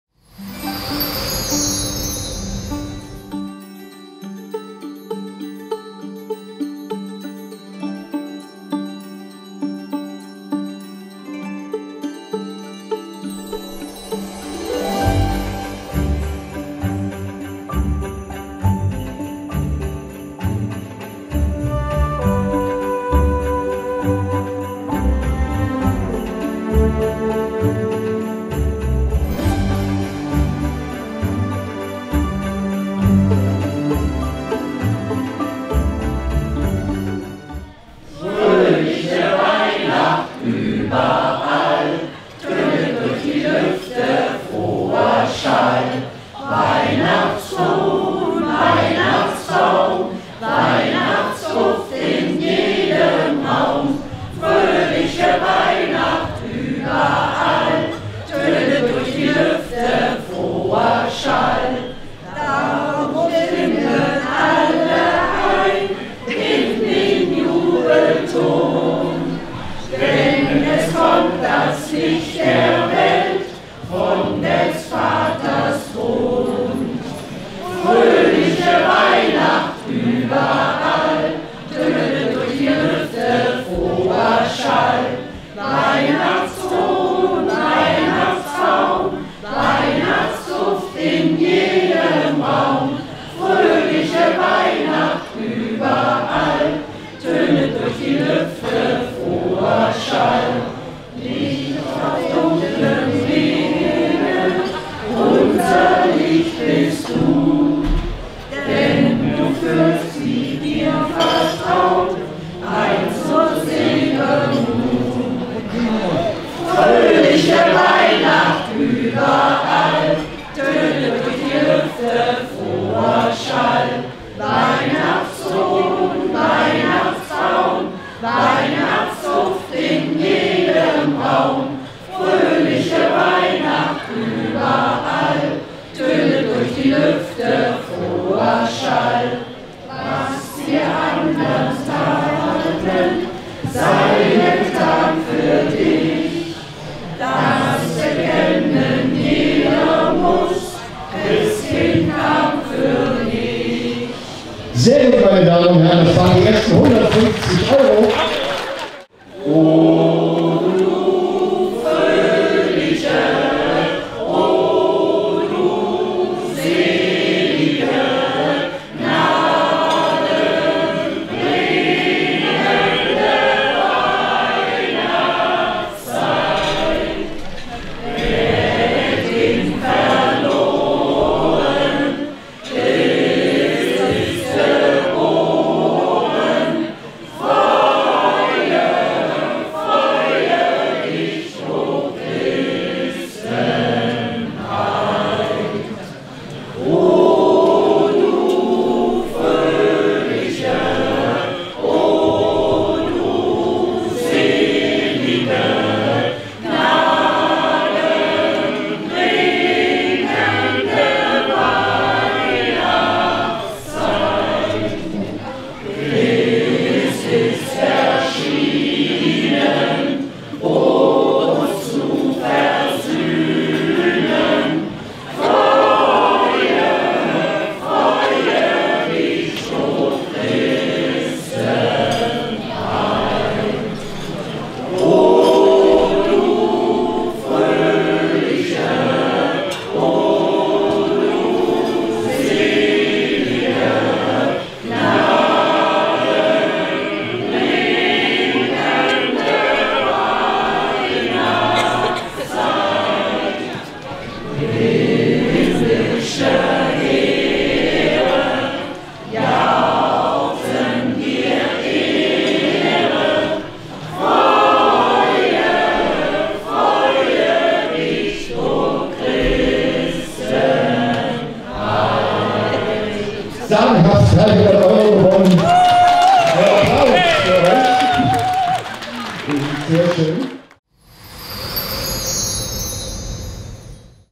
Wintertour 2023 - Frankenau Kellerwald Weihnachtsmarkt
In der Adventszeit brachte die WLZ-HNA-Wintertour festlichen Glanz in den Landkreis. Anna Trebing von der HNA Frankenberger Allgemeine, Patrick Böttcher und Kameramann Christian Wolf bereicherten mit ihrem Besuch die Weihnachtsmärkte der Region. An ihrem Wintertour-Mobil lockten Verlosungen mit attraktiven Preisen wie Einkaufskörben der Waldeck-Frankenberger Bank und einem Wohnmobil-Wochenende von Caravan Konrad.
Ein besonderes Erlebnis war das gemeinsame Singen im Kerzenschein, das nicht nur für leuchtende Augen sorgte, sondern auch 300 Euro für die Vereinskasse einbrachte.
Die Tour hinterließ unvergessliche Momente der Besinnlichkeit und Freude in den Herzen der Besucher.